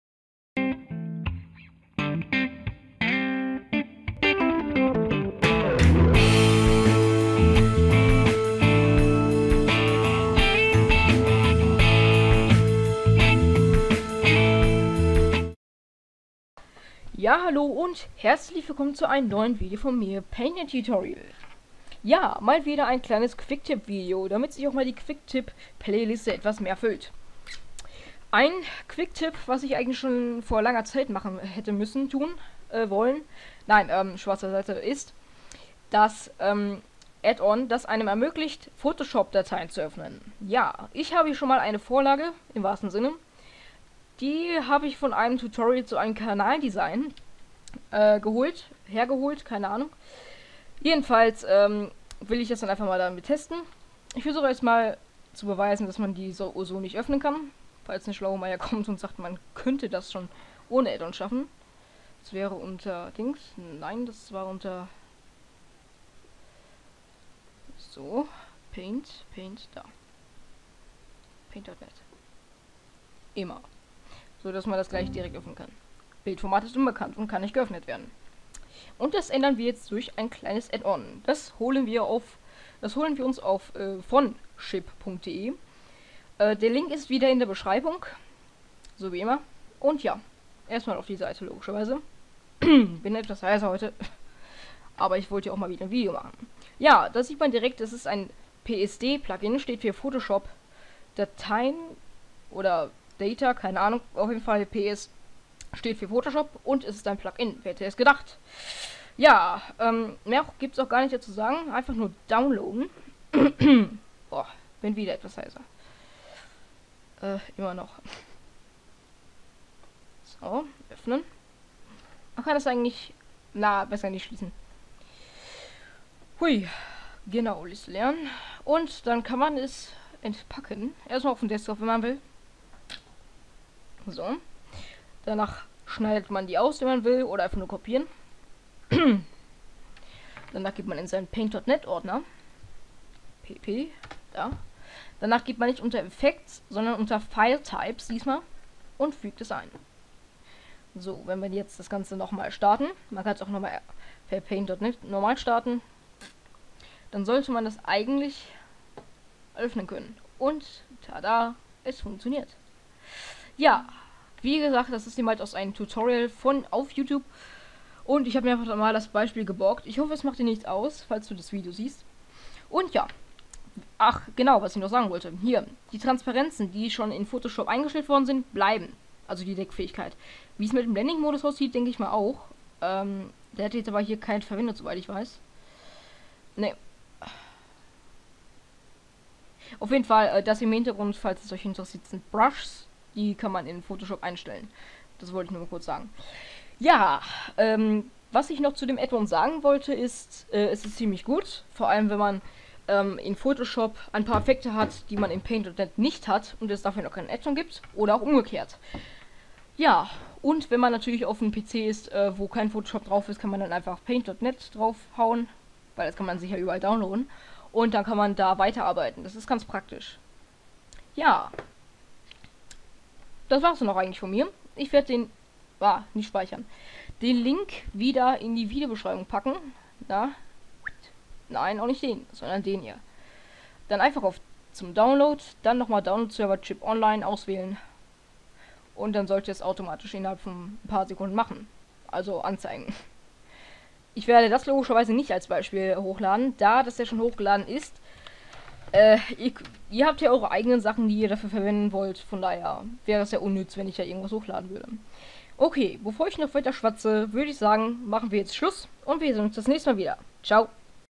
Paint.NET: Photoshop Dateien - Quicktipp [HD]
Ja, hier zeige ich wie man Photoshop Dateien öffnen kann und wozu man sowas machen können tun sollte ( müssen :p ).